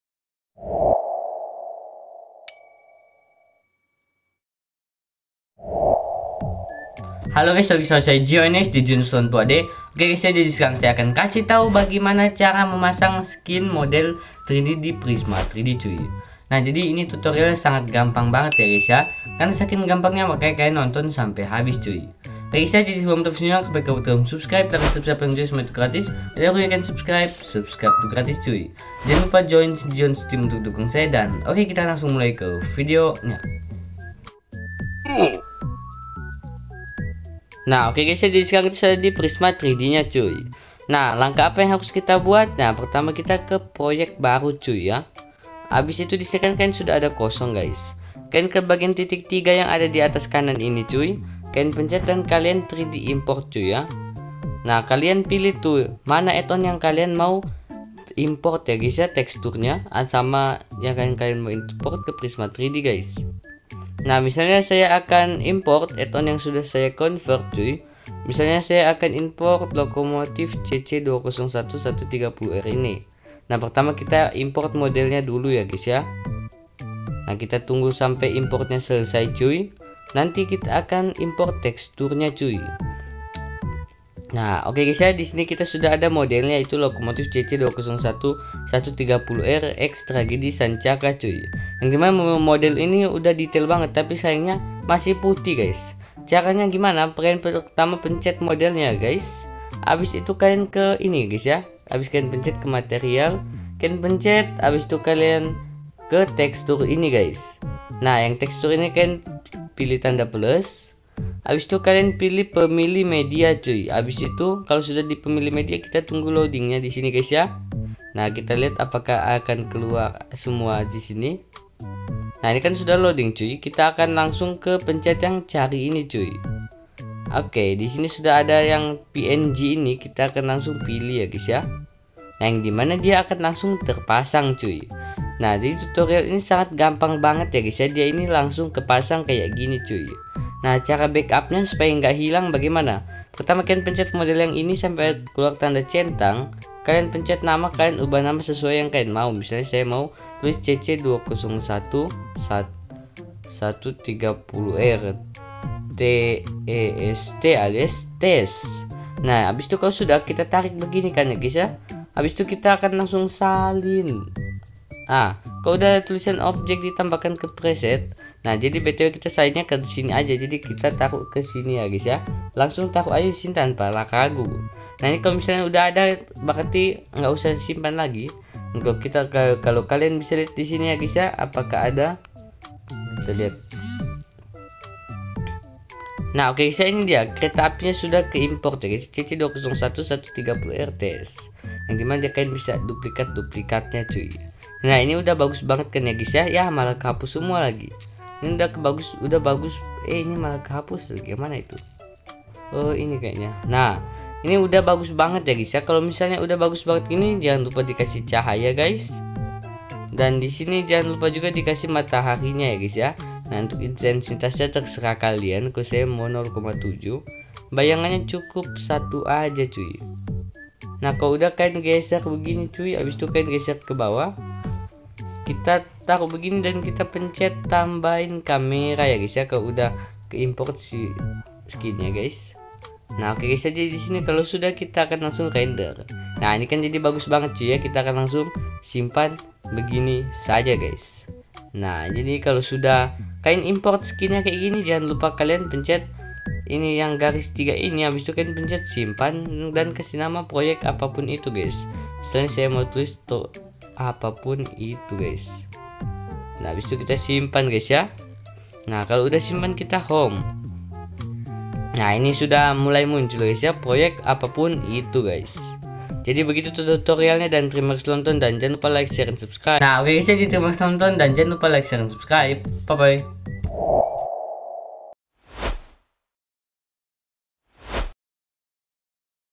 Cara Memasang Skin Model 3D di Prisma 3D Animator
Halo gess balik lagi sama Saya Jions di JIONS1080HD. Nah guys jadi sekarang kita akan kasih tau cara memasang Skin Model 3D di Prisma 3D Animator

📁 Download

⚒️Tutorial Memasang Add-on

❗PERHATIAN
Untuk Menghindari Lokomotif terpisah saat dimainkan, letakan Lokomotif di bagian terakhir Penamaan Trainset pada GRTC, Contohnya Trainset 1-3 Gerbong Kereta dan Trainset 4 adalah Lokomotif

minecraft, train, add-on kereta, indonesia, tutorial, jions

-JIONS1080HD-